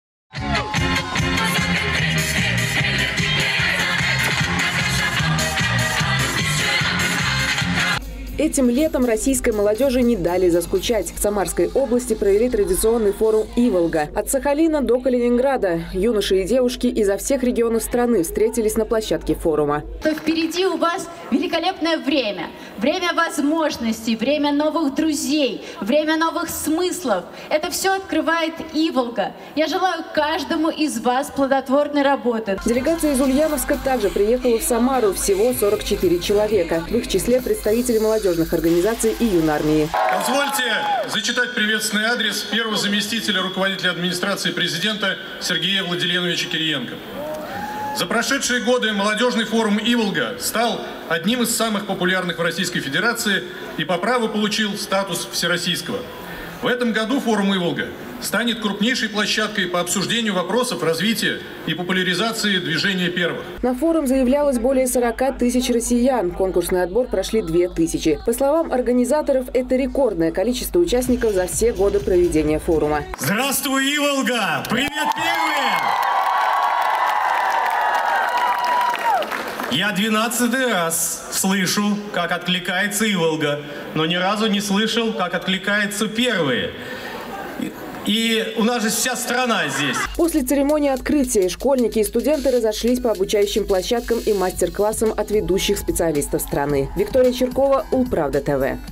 «iВолга» приняла Движение Первых. Ульяновцы отправились на фестиваль в Самару
Молодёжный фестиваль «iВолга» проходит в Самарской области уже в 12-й раз. Теперь у форума статус Всероссийского, он объединился с Движением Первых. В Самаре побывала и делегация из Ульяновска.